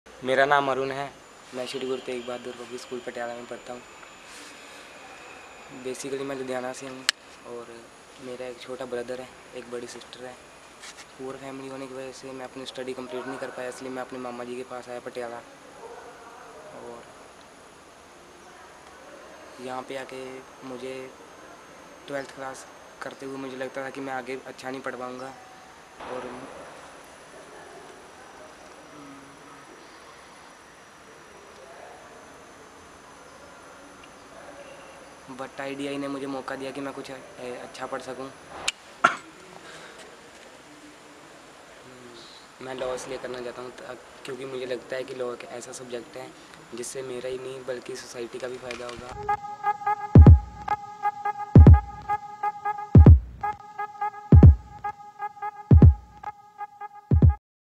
Arun Kumar
Arun is an IDIA Trainee hailing from Ludhiana who is living away from his family with his Uncle for the sake of his education. He wants to study law as it will benefit not only him, but also the society.

Now, only the high application fees stand in the way of his shot at legal education. Help students like Arun by sharing and contributing.